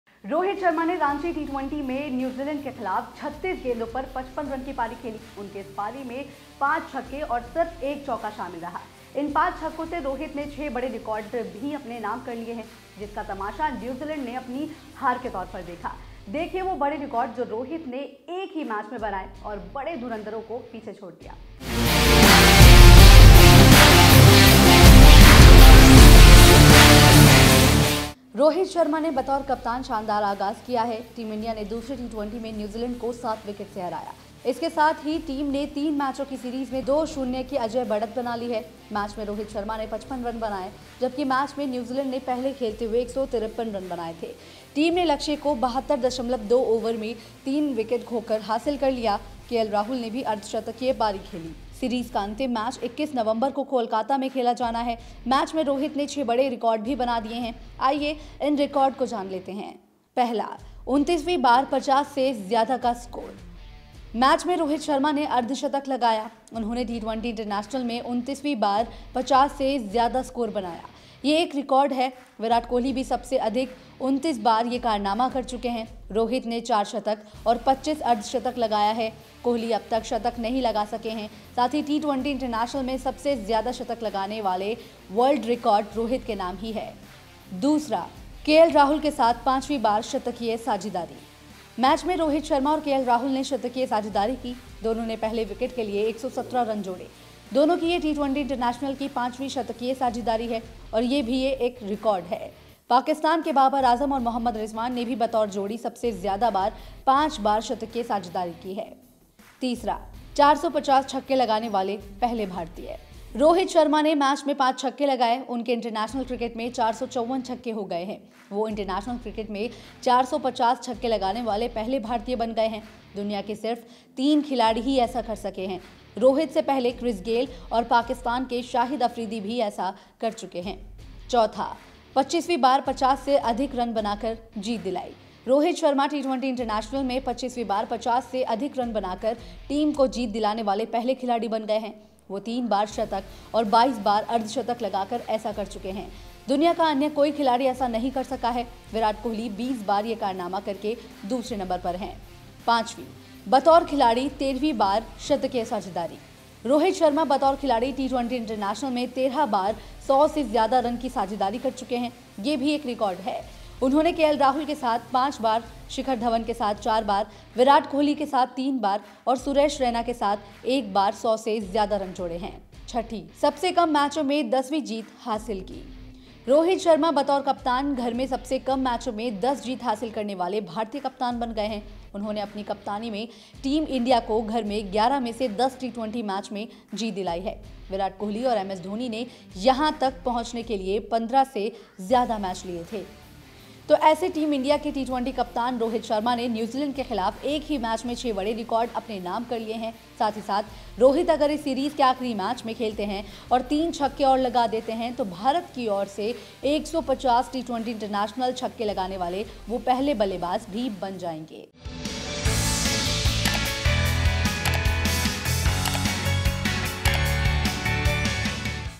IND vs NZ: रोहित शर्मा ने एक मैच में बना दिए 6 बड़े रिकॉर्ड, बाबर आजम-कोहली की बराबरी, धोनी पीछे छूटे
रोहित शर्मा ने रांची T20 में न्यूजीलैंड के खिलाफ 36 गेंदों पर 55 रन की पारी खेली. उनकी इस पारी में 5 छक्के और सिर्फ 1 चौका शामिल रहा. इन 5 छक्कों से रोहित ने 6 बड़े रिकॉर्डों से अपना नाता जोड़ा, जिसका तमाशा न्यूजीलैंड ने अपनी हार के तौर पर देखा… देखिए वो बड़े रिकॉर्ड जो रोहित ने एक ही मैच में बनाए और बड़े धुरंदरो को पीछे छोड़ दिया ..